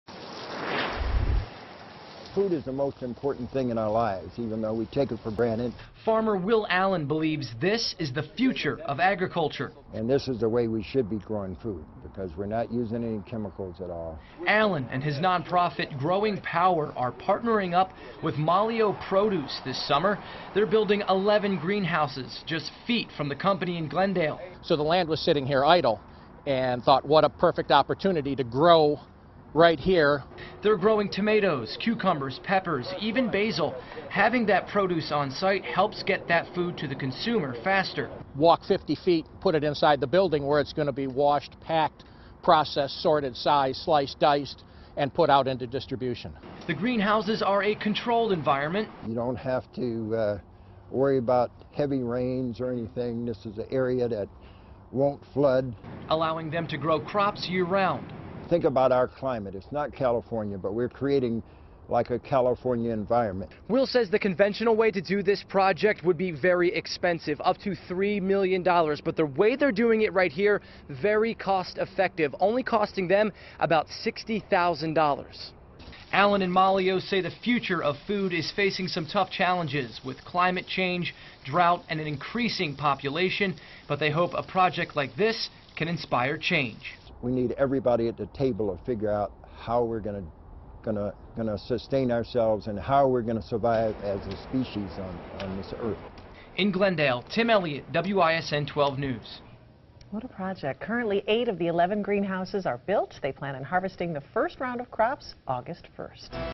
Growing Power, Maglio Produce join forces to build greenhouses
Maglio Produce is working with Growing Power to grow some of its own fruits and vegetables.